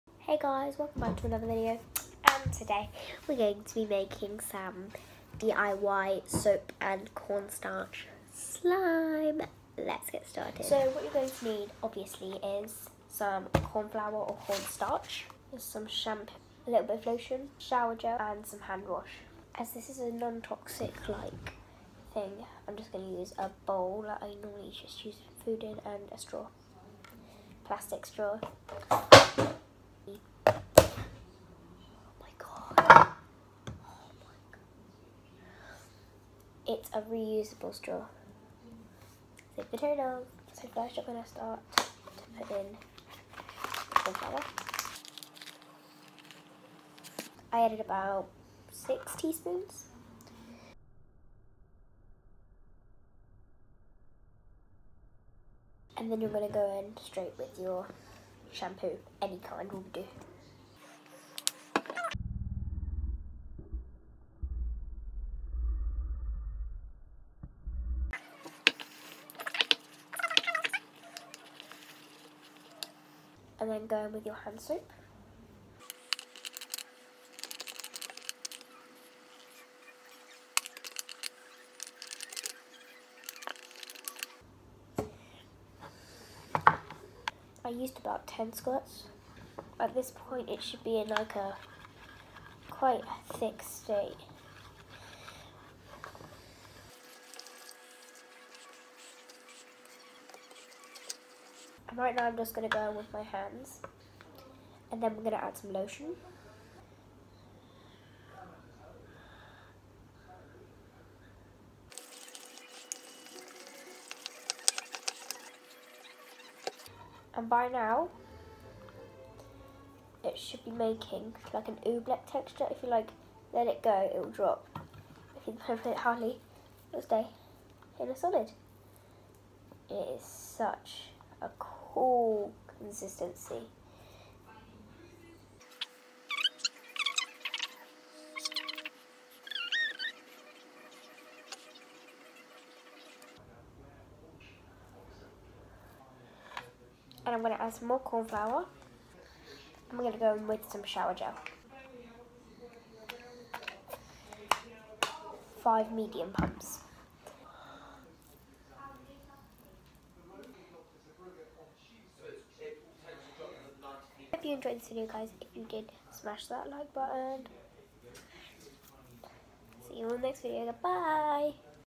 DIY CORNFLOUR AND SOAP SLIME 🧼
This video was a quick do it yourself fun video to do at home. Tonight there might be another video! Bye guys xx